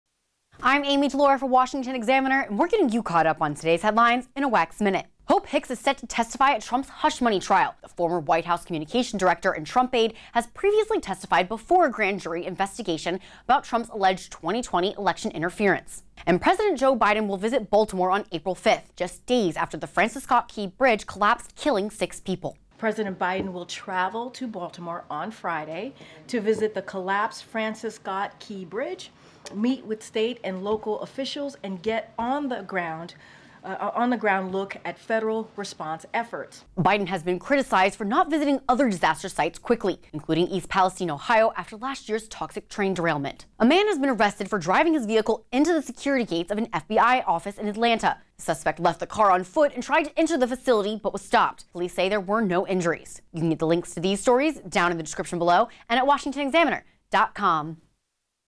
WEX MIN: Hope Hicks testify in Trump trial, Biden to visit Baltimore bridge, FBI building
Hope Hicks is set to testify at Trump’s hush money trial. A former White House communications director and Trump aide, Hicks has previously testified before a grand jury investigation about Trump’s alleged 2020 election interference and said she will testify at the Manhattan trial

President Joe Biden will visit Baltimore on April 5th, just days after the Francis Scott Key Bridge collapsed killing 6 people. Biden has been criticized for not visiting other disaster sites quickly, including East Palestine, Ohio, after last year’s toxic train derailment.

A man has been arrested for driving his vehicle into the security gates of an FBI office in Atlanta. The suspect left the car on foot and tried to enter the facility but was stopped. Police say there were no injuries.